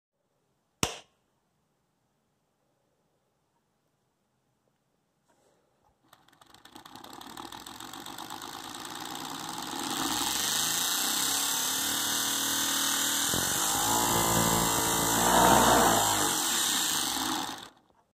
Peak 4500 RPM Homemade DC Motor
Homemade DC motor peak 4500 RPM Tachometer tested.